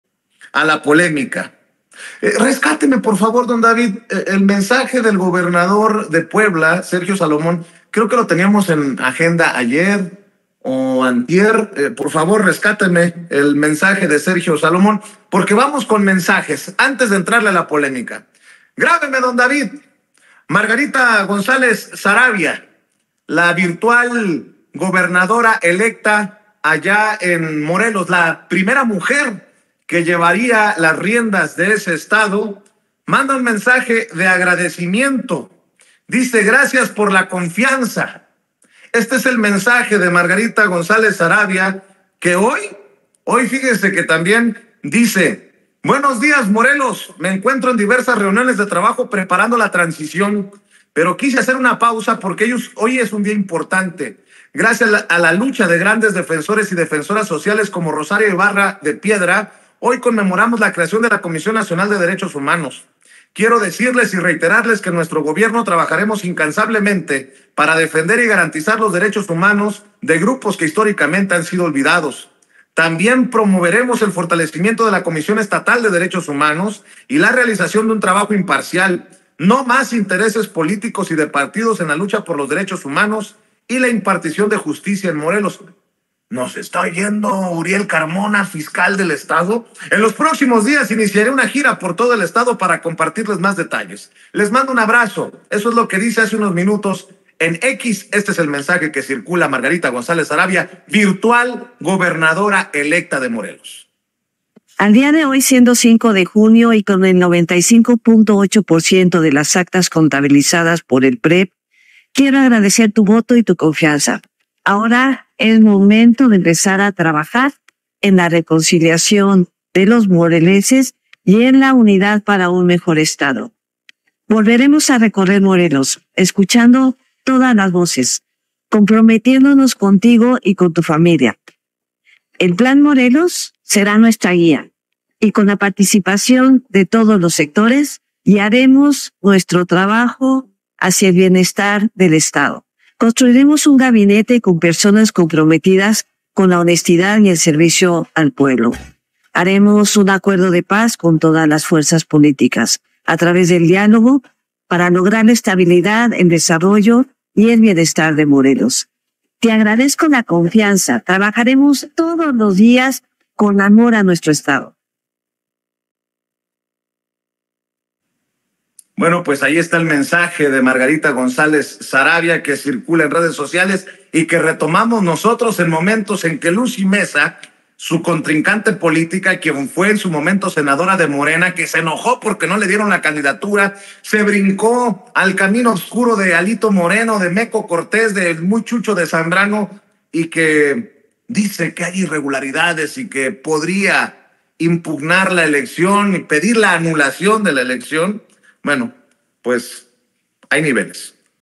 MARGARITA AGRADECE CONFIANZA A MORELENSES Y RECALCA QUE TRABAJARÁ POR DERECHOS HUMANOS
La virtual gobernadora electa en Morelos, Margarita González Saravia, envía un mensaje de agradecimiento al Pueblo que le otorgó su confianza. Además, aprovecha el marco de la celebración de la creación de la Comisión Nacional de Derechos Humanos y puntualiza que en su gobierno se trabajará incansablemente para defenderlos.